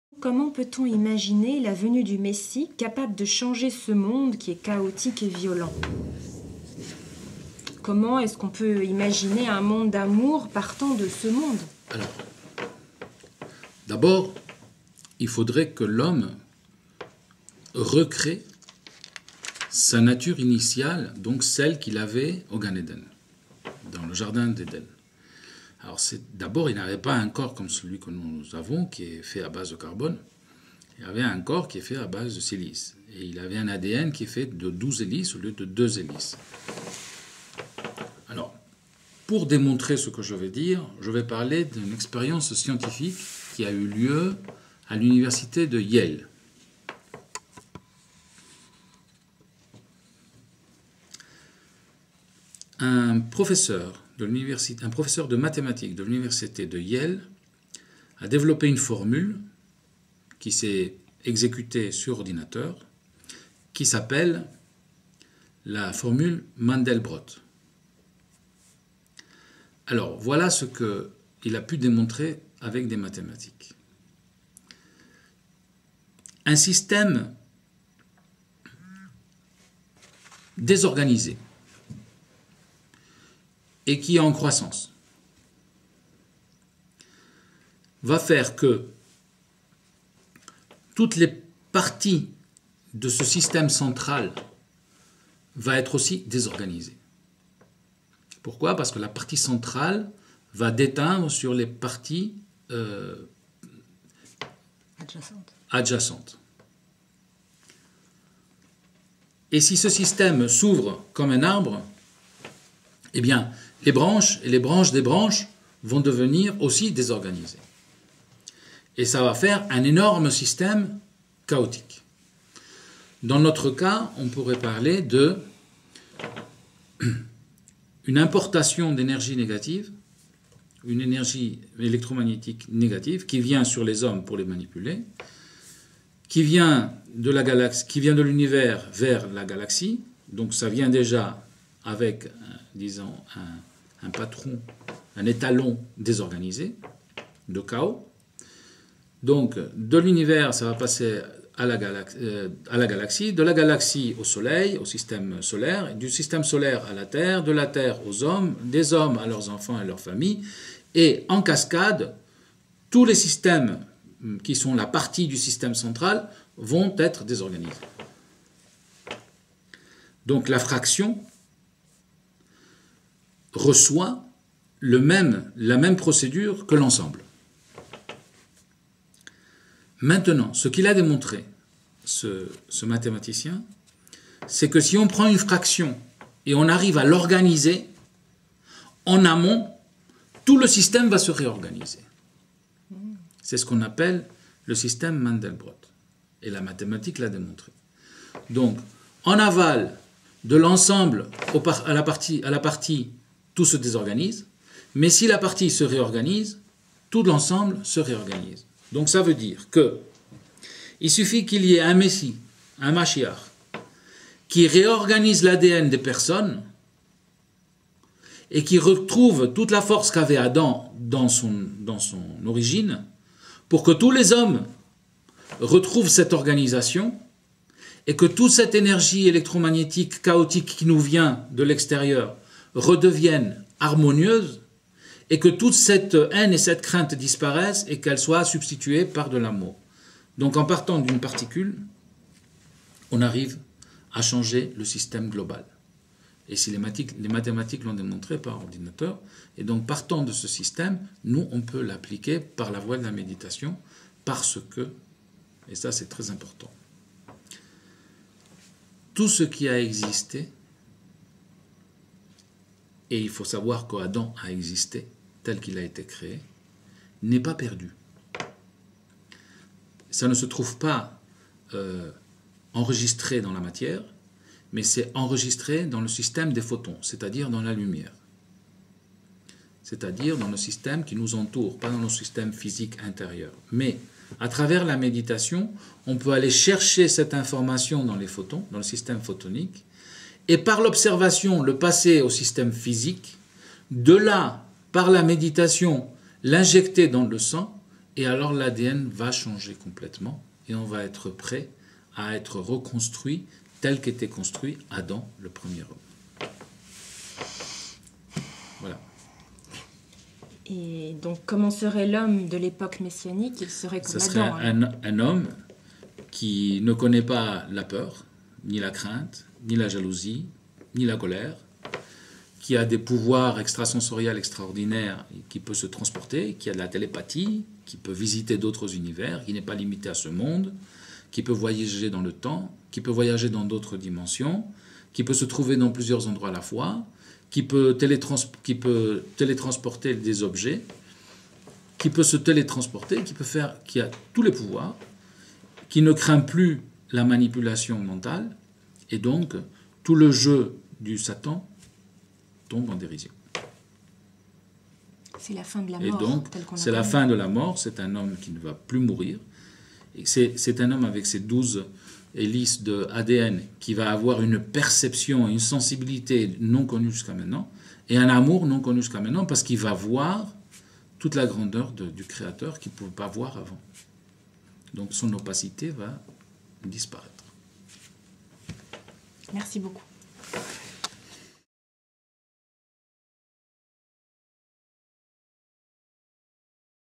La venue du Messie
Albert Gozlan, Maître de Kabbalah, répond à la question: Comment peut-on imaginer la venue du Messie capable de changer ce monde chaotique et violent? Comment peut-on imaginer un monde d'amour partant de ce monde?